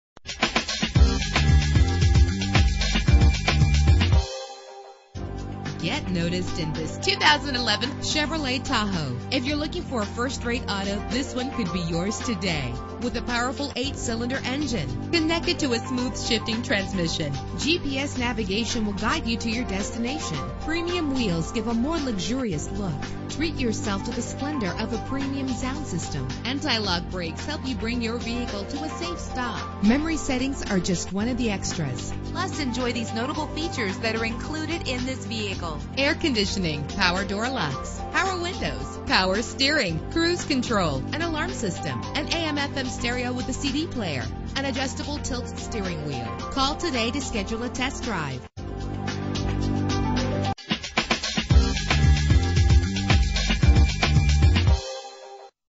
2011 Chevrolet Tahoe - New Canaan CT
Year: 2011
Make: Chevrolet
Model: Tahoe
Trim: Hybrid
Engine: 6.0 liter 8 cylinder 16 valve
Transmission: 2
Color: Mocha Steel Metallic
Mileage: 4
Address:
261 Elm Street